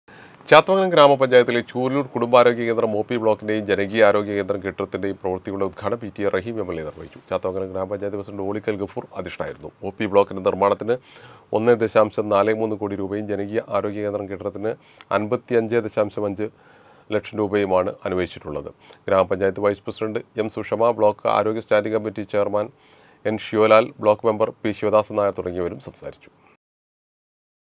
ചാത്തമംഗലം ഗ്രാമപഞ്ചായത്തിലെ ചൂലൂർ കുടുംബാരോഗ്യ കേന്ദ്രം ഒ.പി ബ്ലോക്കിന്റെയും ജനകീയ ആരോഗ്യ കേന്ദ്രം
ചാത്തമംഗലം ഗ്രാമപഞ്ചായത്തിലെ ചൂലൂർ കുടുംബാരോഗ്യ കേന്ദ്രം ഒ.പി ബ്ലോക്കിന്റെയും ജനകീയ ആരോഗ്യ കേന്ദ്രം കെട്ടിടത്തിന്റെയും പ്രവൃത്തികളുടെ ഉദ്ഘാടനം പി.ടി.എ റഹീം എംഎൽഎ നിർവഹിച്ചു